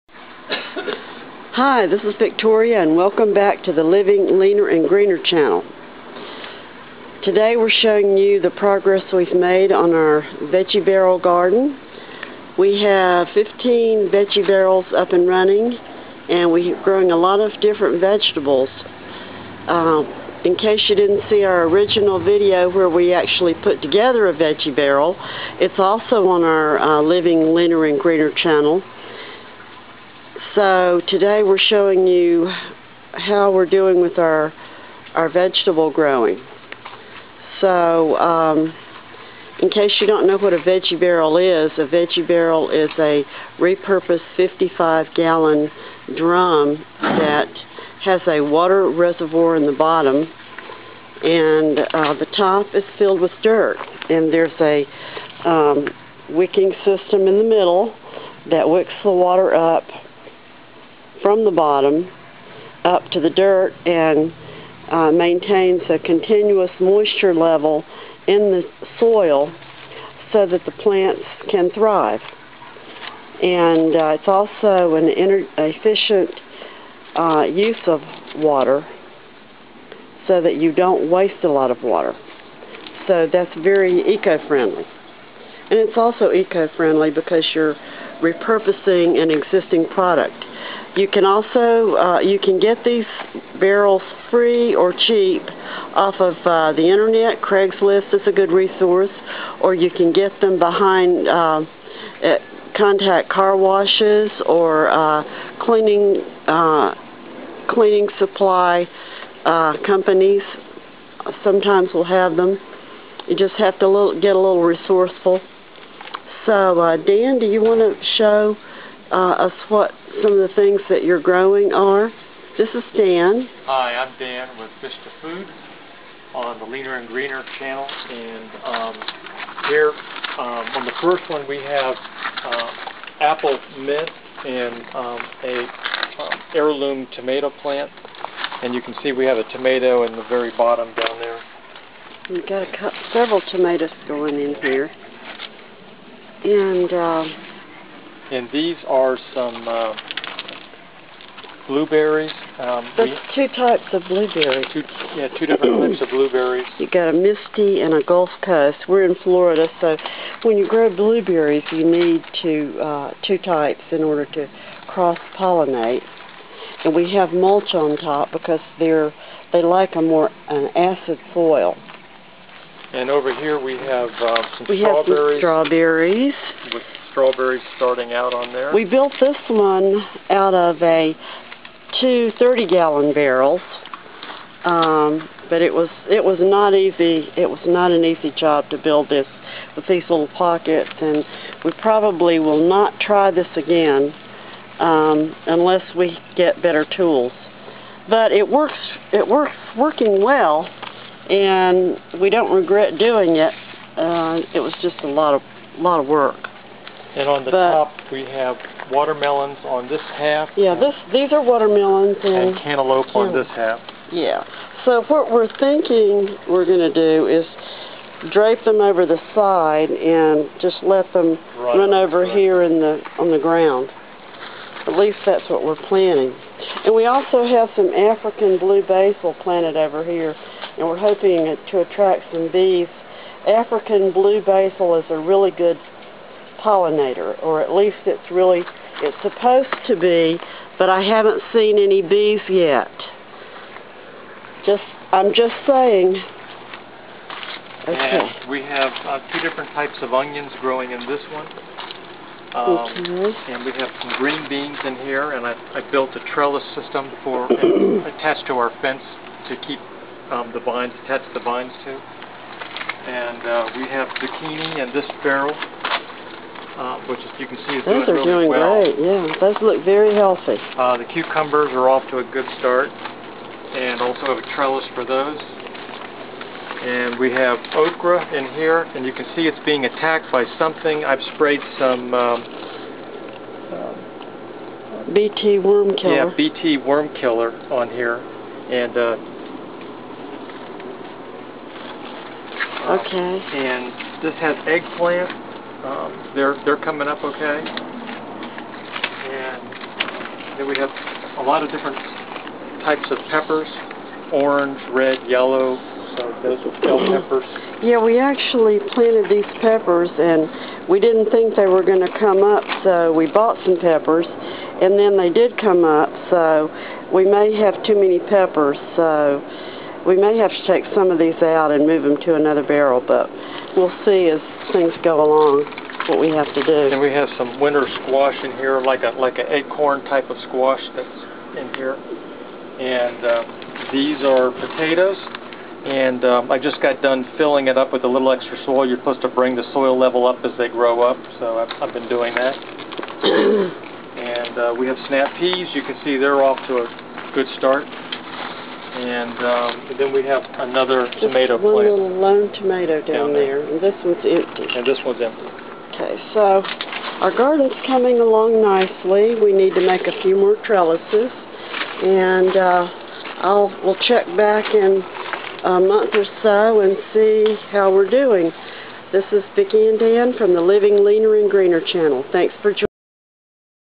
VEGGIE BARRELS APRIL.avi
Veggie Barrels are self-watering containers with a wicking systems that draws water up from the bottom water reservoir. Soil is added to the top of the barrel. These barrels are made from repurposed HDPE 55-gallon barrels. Our garden was planted in March so this is a few weeks of growth.